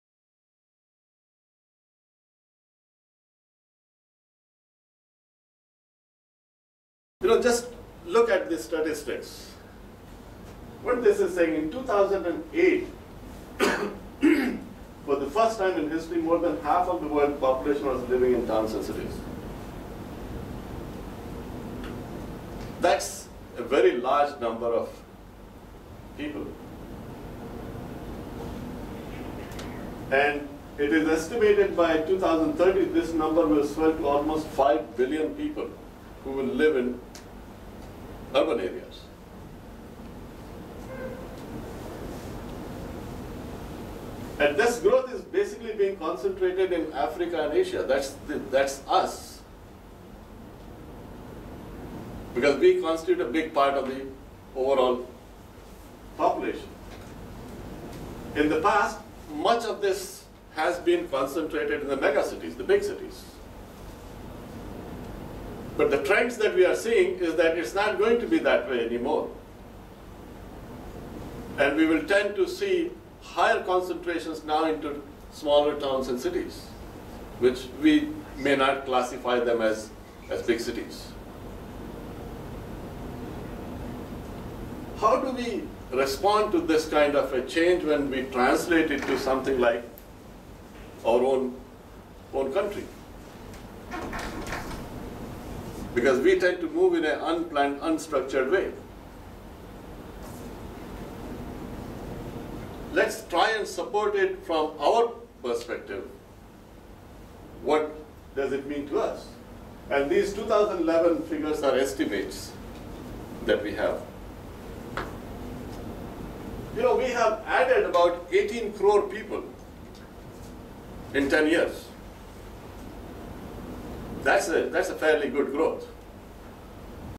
Surat on the Rise (Part 2)
Real Surat Development Seminar (RSDS-11), Dr. Kamlesh Misra, Vice Chancellor, Auro University